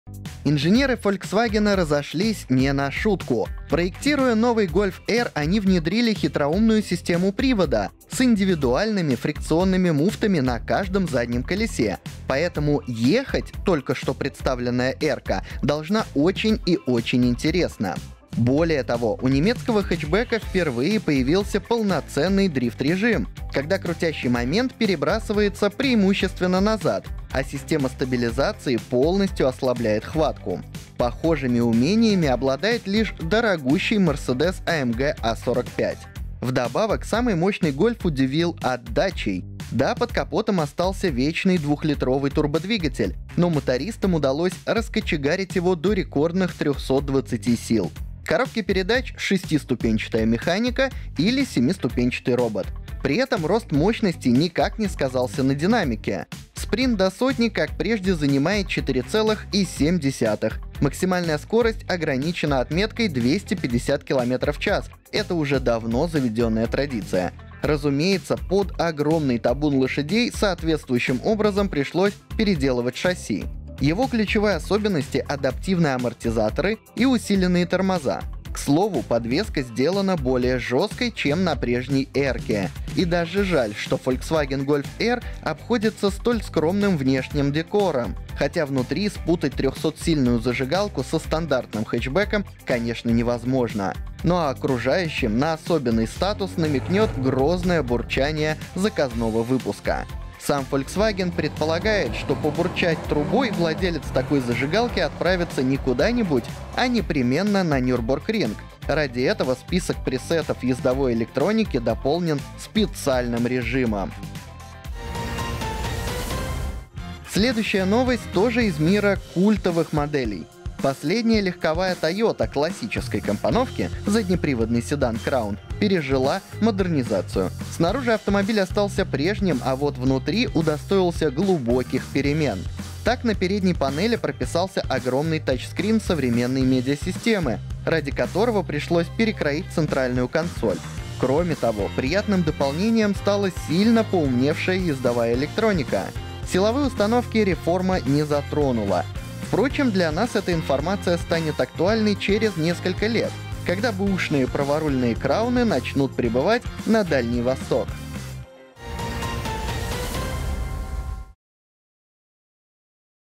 Volkswagen Golf R готов прохватить боком. Toyota Crown с новым салоном | Новости с колёс №1207
Инженеры Фольксвагена разошлись не на шутку! Проектируя новый Golf R, они внедрили хитроумную систему привода — с индивидуальными фрикционными муфтами на каждом заднем колесе. Поэтому ехать только что представленная «эрка» должна очень и очень интересно. Вдобавок самый мощный Гольф удивил отдачей. Да, под капотом остался вечный двухлитровый турбодвигатель, но мотористам удалось раскочегарить его до рекордных 320 сил!

Последняя легковая Toyota классической компоновки, заднеприводный седан Crown, пережила модернизацию. Снаружи автомобиль остался прежним, а вот внутри удостоился глубоких перемен.